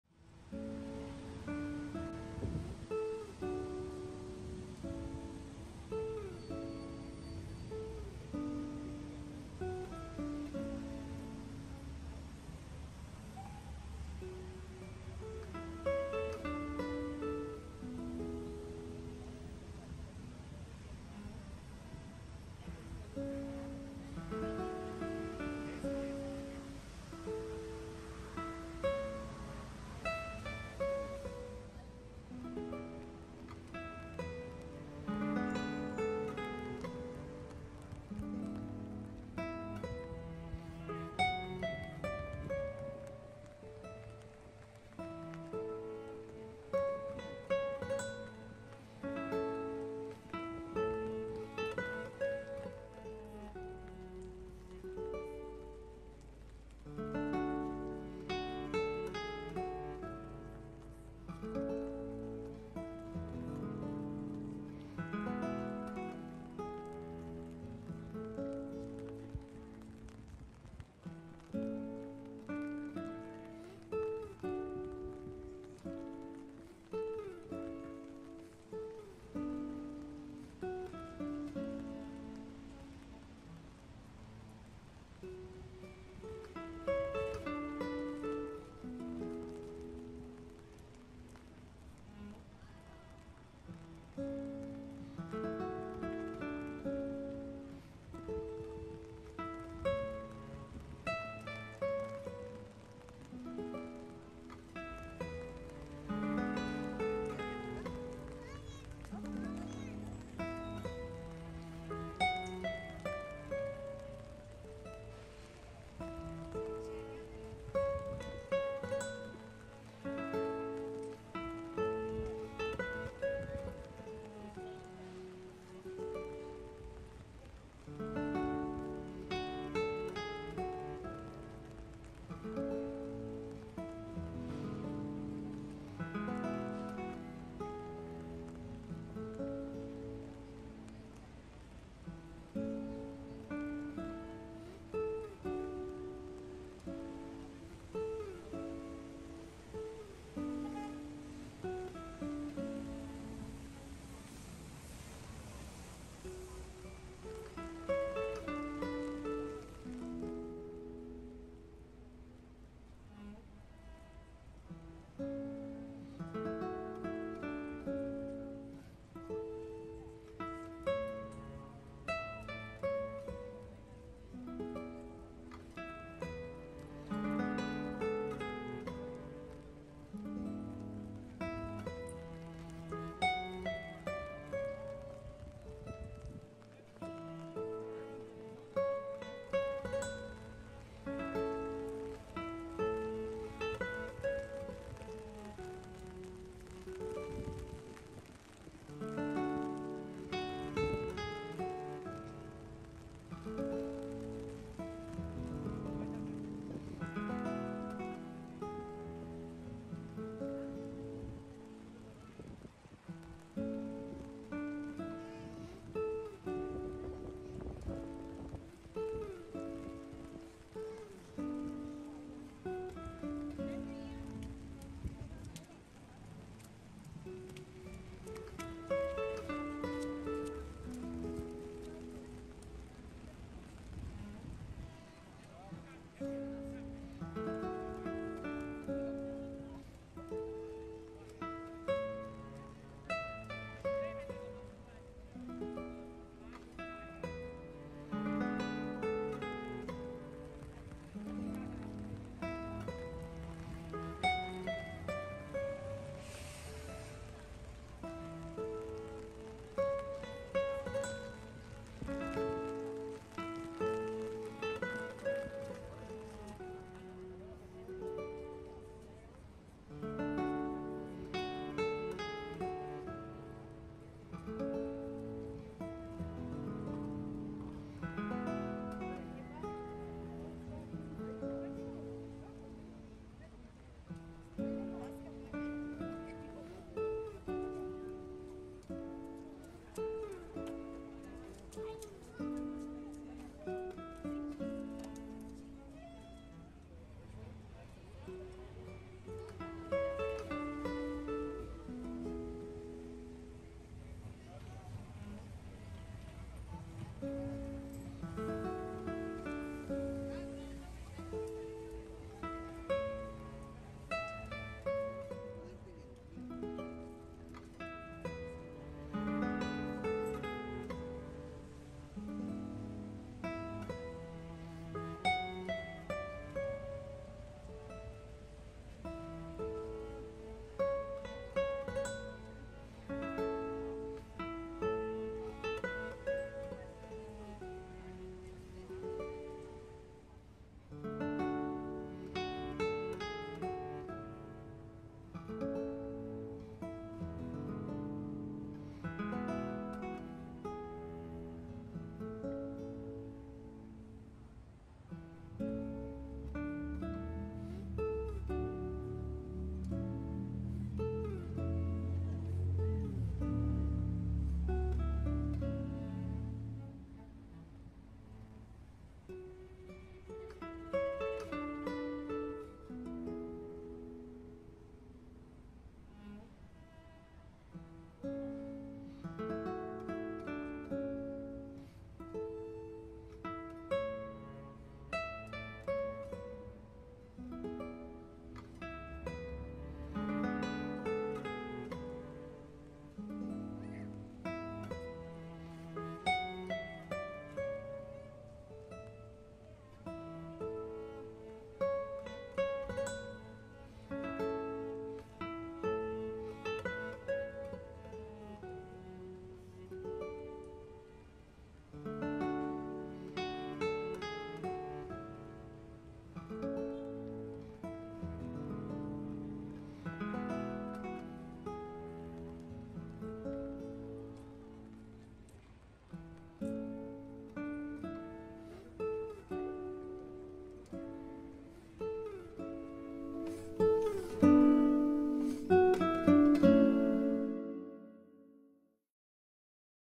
Exposición Día de Muertos en New York
Día de Muertos, una hermosa tradición Mexicana adorna el Rockefeller Center de New York.
Aquí te comparto con orgullo unas imágenes, espero las disfrutes.